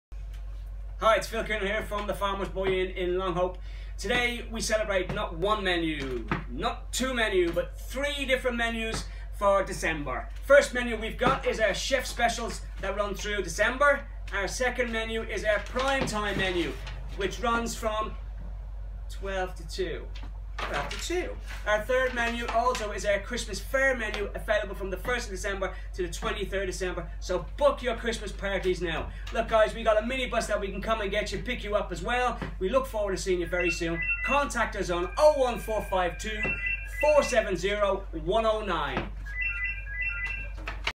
Not 1, Not 2 but 3 New Menus Launched Today!
We’ve gone all out on the menu launches today!
New Chef’s Specials available throughout December
Senior Citizens Prime Tine Menu Monday-Thursday 12-2pm
Christmas Fayre Menu available for Christmas Parties